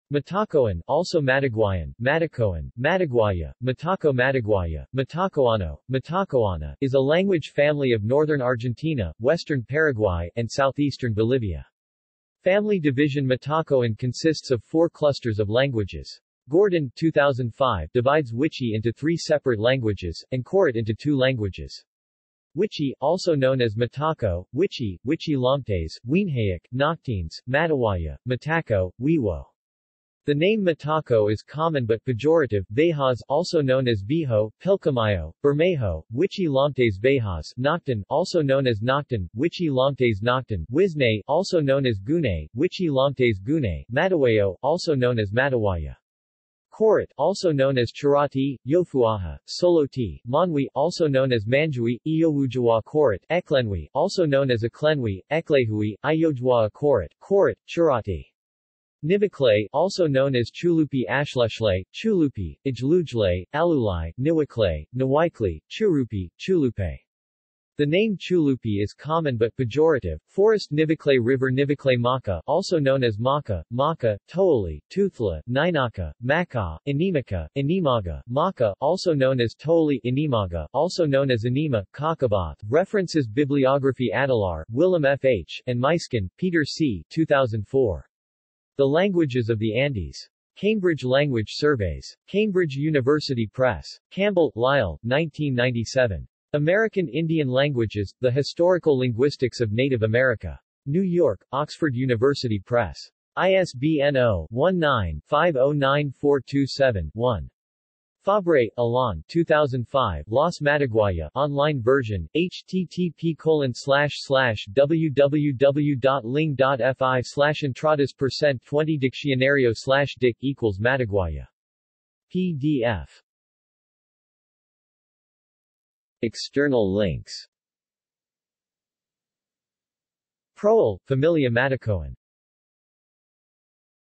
Matacoan languages | Wikipedia audio article
This is an audio version of the Wikipedia Article:
Matacoan languages

SUMMARY
Matacoan (also Mataguayan, Matákoan, Mataguayo, Mataco–Mataguayo, Matacoano, Matacoana) is a language family of northern Argentina, western Paraguay, and southeastern Bolivia.